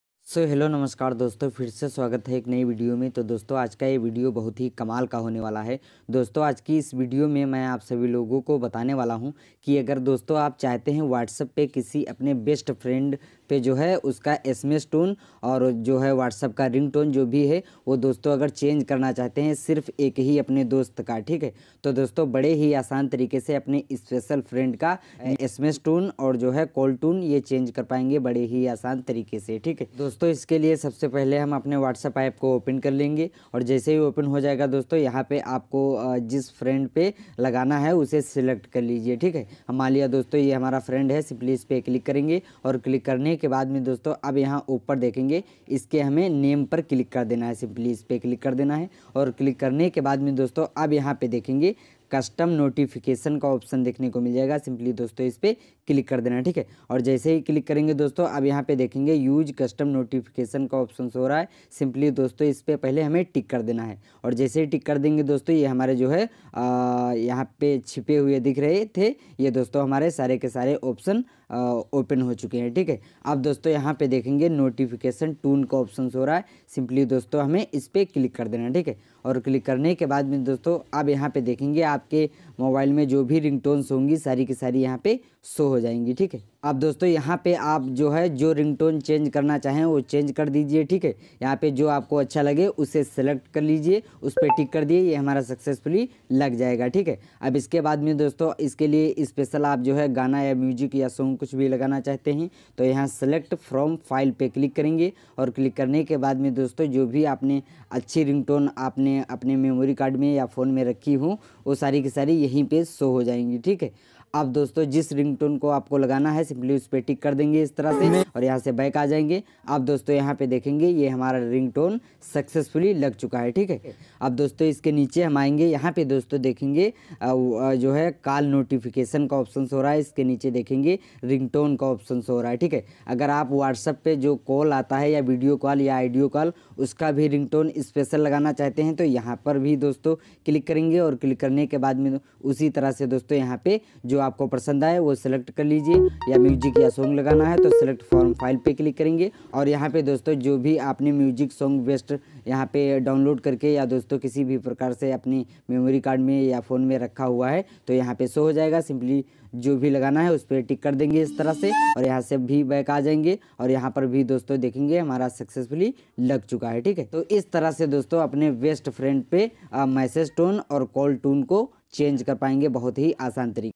WhatsApp me Apne best friend per special tune Kaise lagaeye / WhatsApp me friend per ringtone lagaey
hello doston aaj ki video mein humne aapko bataya WhatsApp mein Apne kisi ek Friend per special tune Kaise lagaen